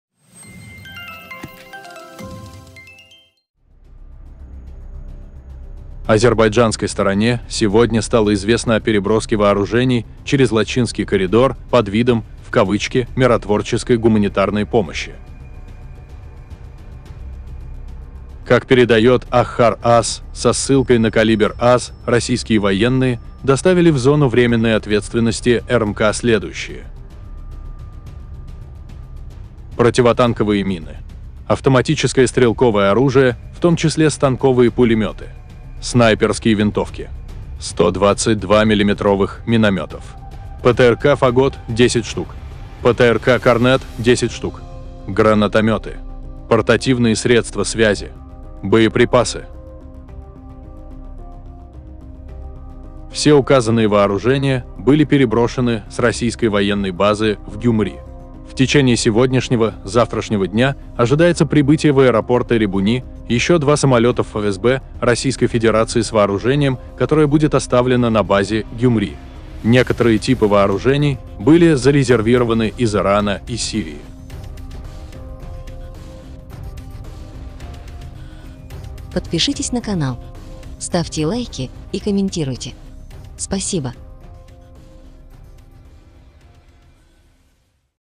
Российские военные вооружают карабахских сепаратистов?
Азербайджанской стороне сегодня стало известно о переброске вооружений через Лачинский коридор под видом «миротворческой гуманитарной помощи».

Как передает Axar.az со ссылкой на Caliber.az, российские военные доставили в зону временной ответственности РМК следующие:
- противопехотные мины
- противотанковые мины
- автоматическое стрелковое оружие, в т.ч. станковые пулеметы
- снайперские винтовки
- минометы 122-мм
- ПТРК «Фагот» (10 шт.)
- ПТРК «Корнет» (10 шт.)
- гранатометы
- портативные средства связи
- боеприпасы
Все указанные вооружения были переброшены с российской военной базы в Гюмри. В течение сегодняшнего/завтрашнего дня ожидается прибытие в аэропорт Эребуни еще 2 самолетов ФСБ РФ с вооружением, которое будет оставлено на базе Гюмри. Некоторые типы вооружений были зарезервированы из Ирана и Сирии.